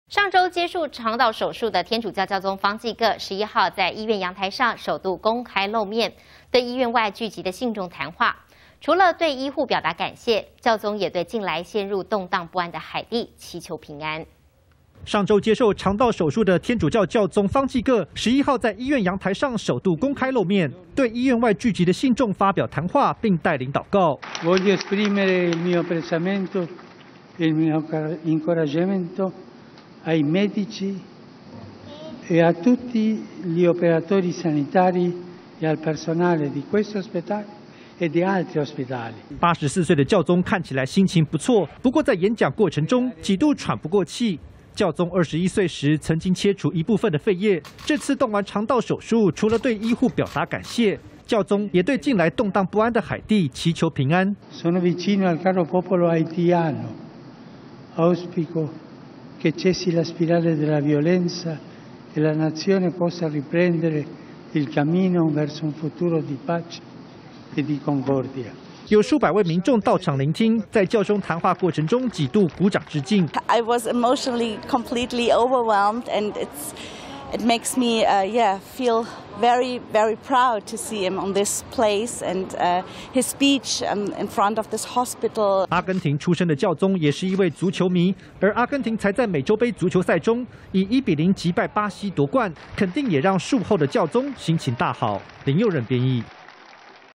術後首度露面談話 教宗方濟各感謝醫護
上周接受腸道手術的天主教教宗方濟各，11號在醫院陽台上首度公開露面，對醫院外聚集的信眾說話，除了對醫護表達感謝，教宗也對近來陷入動盪不安的海地祈求平安。